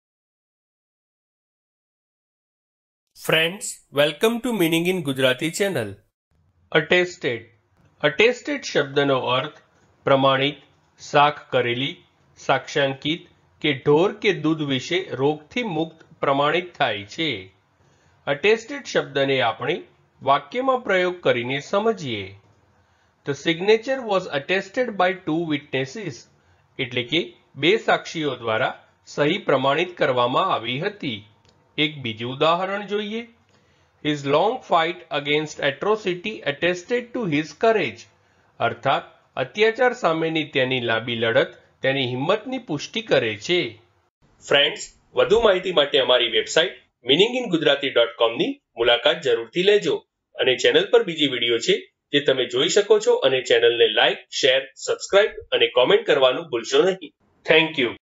Attested Meaning in Gujarati | Attested નો અર્થ શું છે | Attested in Gujarati Dictionary
આ વિડિયો માં તમે Attested નો Gujarati માં અર્થ સમજશો અને એની સાથે Attested નું pronunciation પણ શિખશો. એટ્લે કે તમે Attested ના meaning ની સાથે સાથે એ પણ શિખશો કે Attested ને કેવી રીતે બોલાય, Attested ને બોલવાની સાચી રીત કઈ છે.

તો બસ એક મિનટ માં શીખો Attested શબ્દ ને.

Attested ને examples થી સમજીએ